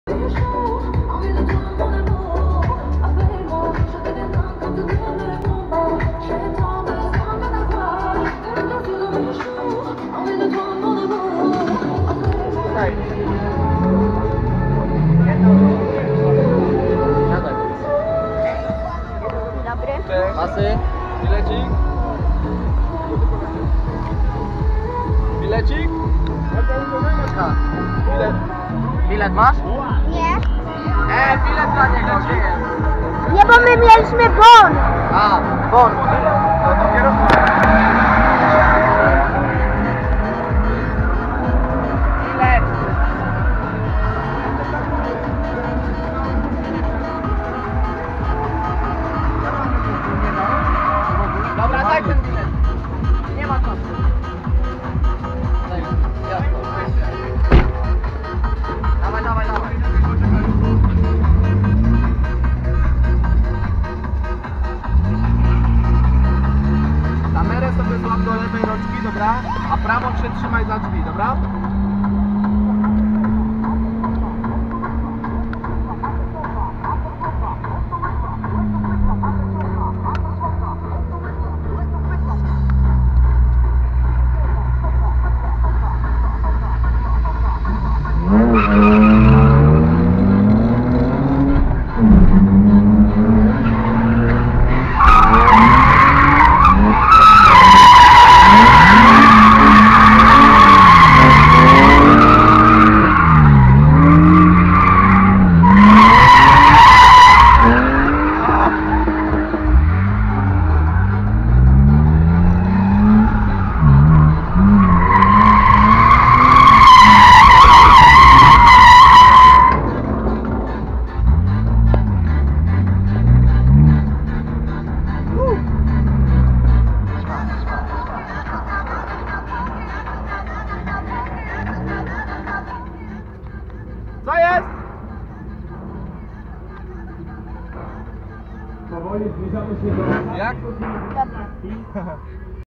MOTO SHOW BIELSKO BIAŁA 2021❗ WYGRAŁEM PRZEJAZD ❗BMW TAXI DRIFT ❗ KRONIKA BESKIDZKA ❗
Witam   Na Moto show w Bielsku Białej wygrałem od Kroniki Beskidziej przejazd taxi Driftem , miałem okazję podriftowac samochodem BMW , oczywiście jako pasażer , zapraszam do oglądania